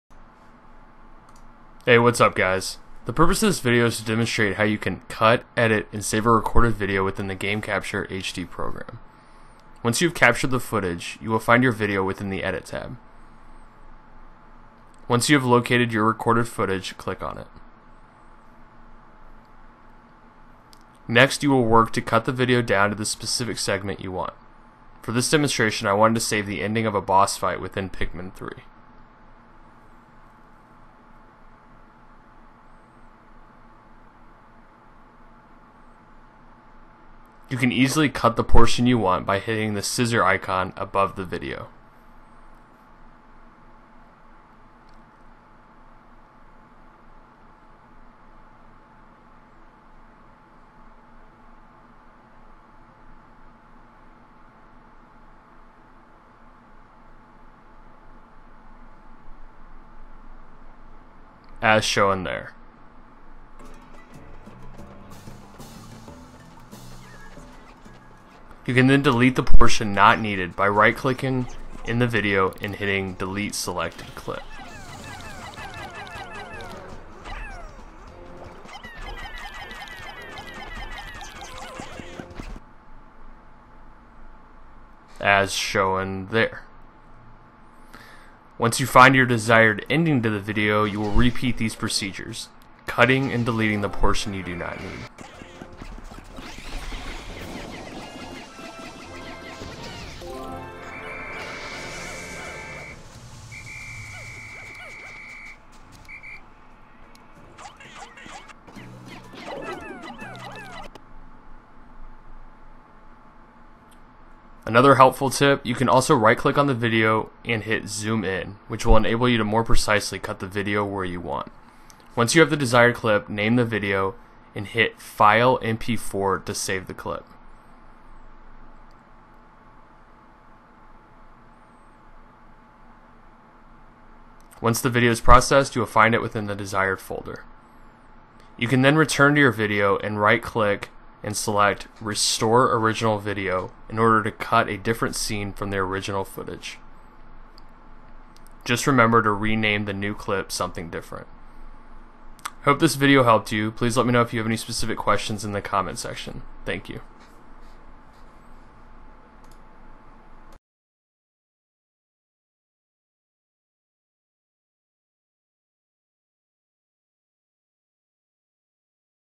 Video Tutorial Guide - How to Cut / Edit / Save Clips Using Elgato Game Capture HD / HD60
Short video demonstrating how to cut, edit, and save desired clips / scenes using the Elgato Game Capture HD / HD60 program.